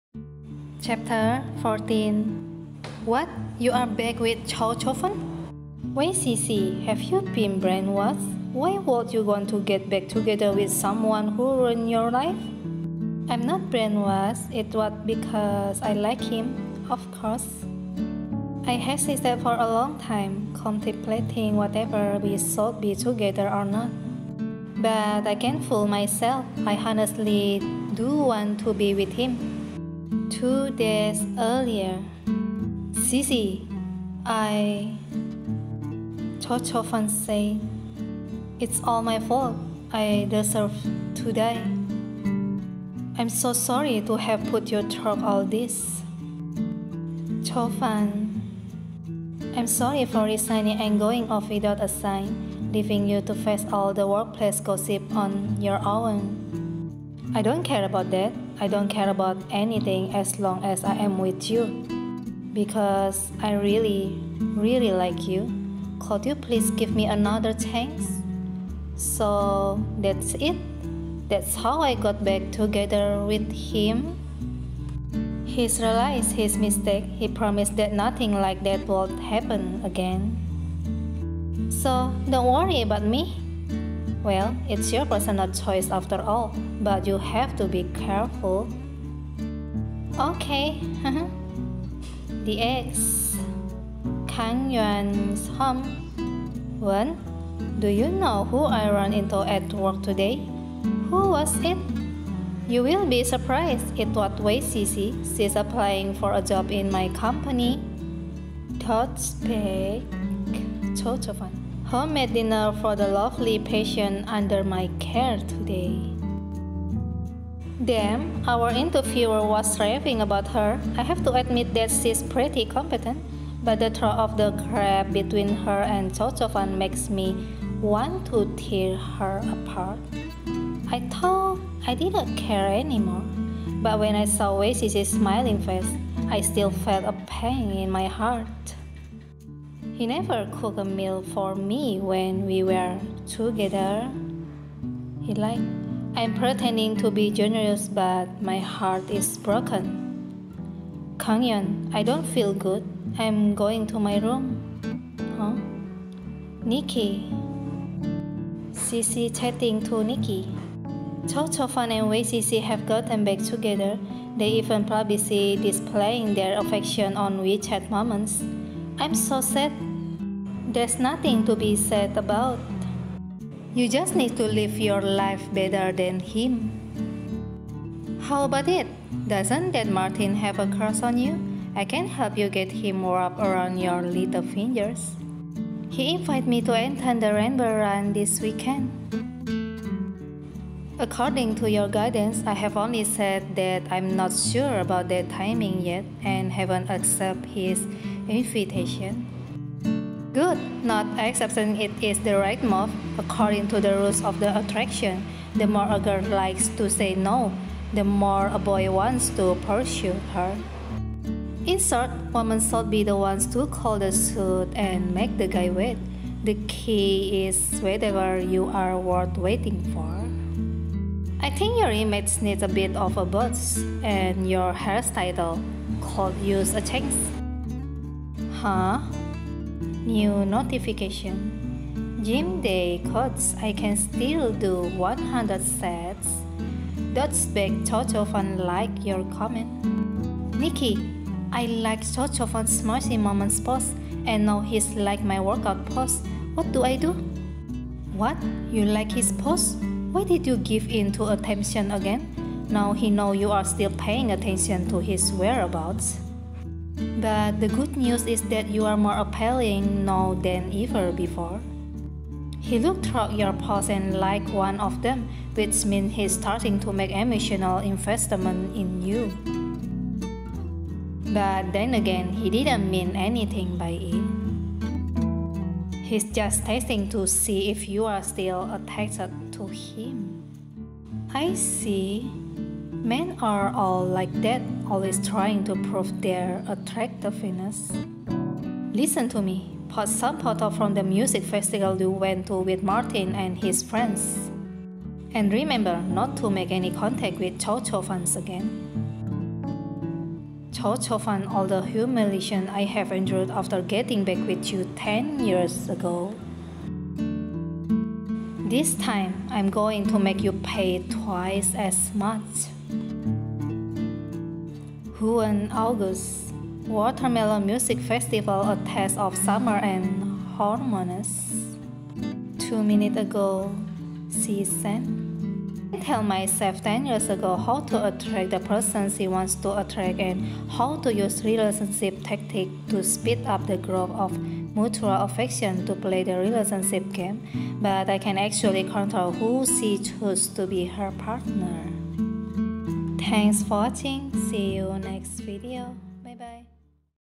The Enemy was my Ex Boyfriends Chapter 14 English Sub
Don't forget to download Mangatoon Application on your Android phone

A mysterious phone call from the future helped her counterattack in love?!

A guideline that will help you get to know more about the

Love, is a war. Bloodless, but leaves you in scars. An ugly girl who was cheated by her ex received a mysterious phone call. Strangely, the other line said that she is 35 years old, and that she is her, from the future! In order to change her, she wants to teach her the guideline of love. For her to see the beauty in her, take revenge on her ex, and recapture everything she lost before.

Manga Toon got authorization from Kuaikan Comics to publish this content, the content is the author's own point of view, and does not represent the stand of Manga Toon.